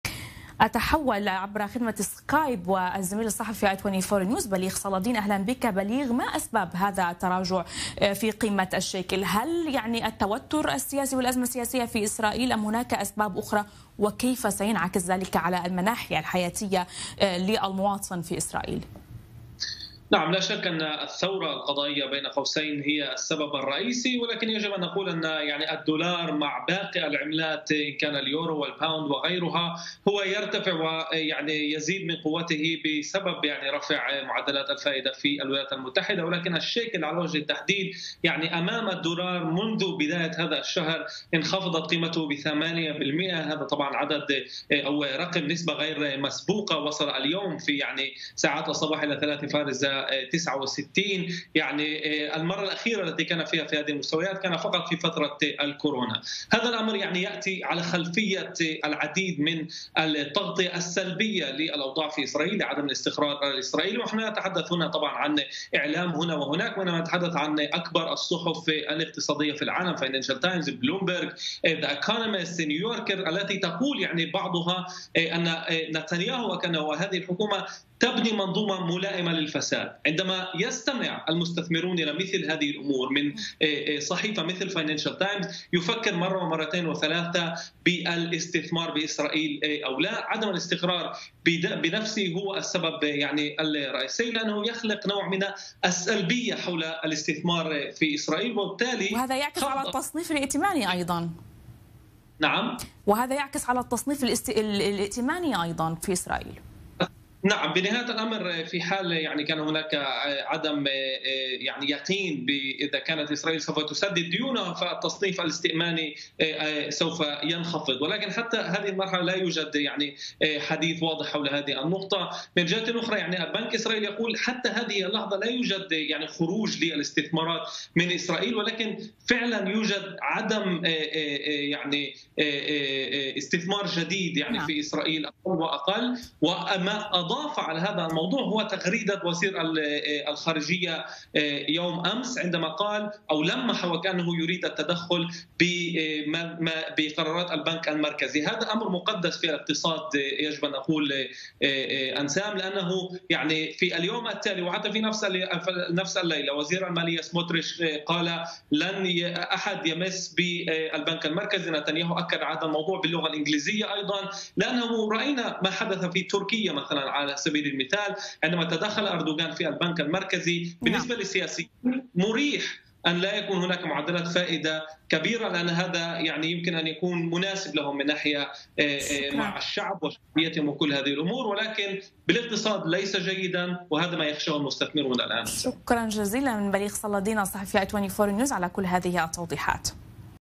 الشيكل الإسرائيلي في أدنى مستوياته: ما هو السبب وكيف يؤثر ذلك على المواطنين؟
الشيكل وصل إلى أدنى مستوى له منذ 3 سنوات بعد تمرير قوانين "الثورة القضائية" بالقراءة التمهيدية والقراءة الأولى في الكنيست.

تقديم: أنسام سلمان

ــــــــــــــــــــــــــــــــــــــــــــــــــــــــــــــــــــــ